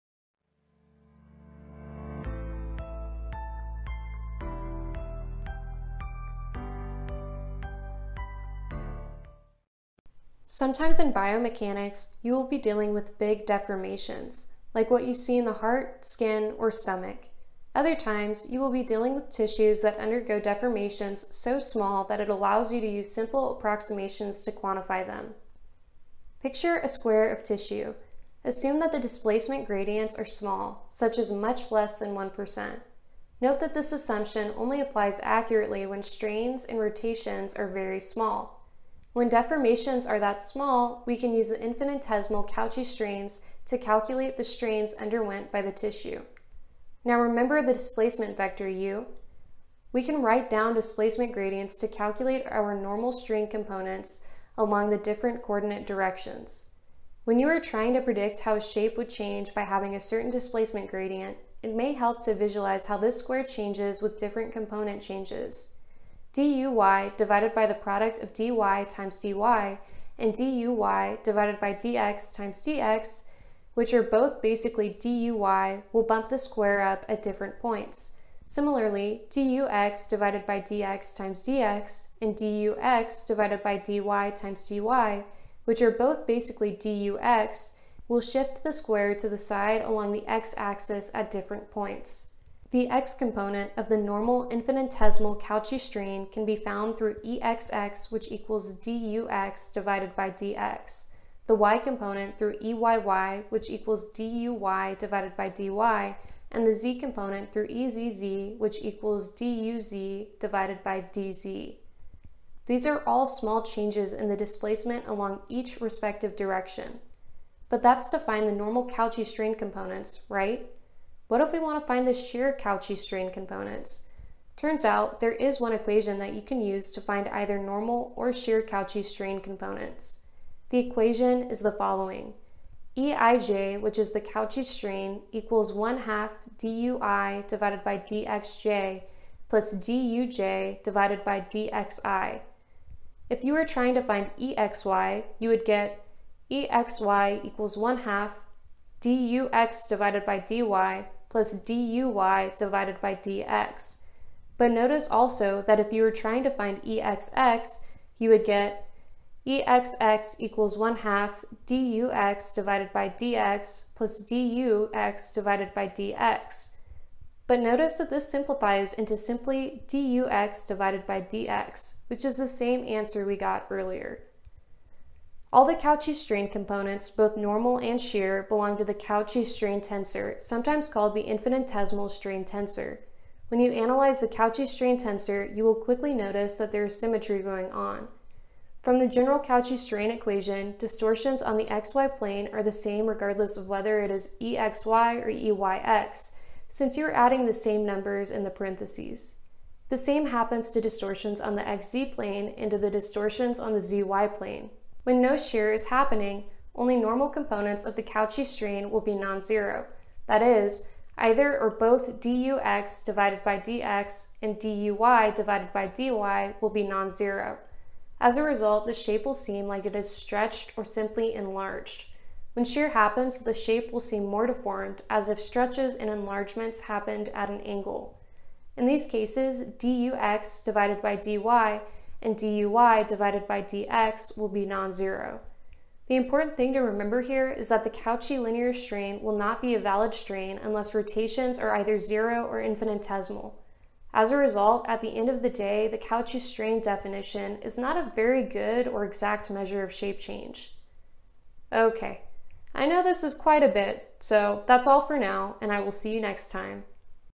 Biomechanics | Infinitesimal Cauchy Strain
When strains are infinitesimally small, you may want to model them using infinitesimal Cauchy strains. Infinitesimal Cauchy strains are not very useful in biomechanics since deformations are, more often than not, not infinitesimal. Nevertheless, they do come up in your courses. Let's check them out! We will also touch on the Cauchy strain tensor in this video.

Want to give us some feedback so that we can better help you? Let us know what you think about the video here: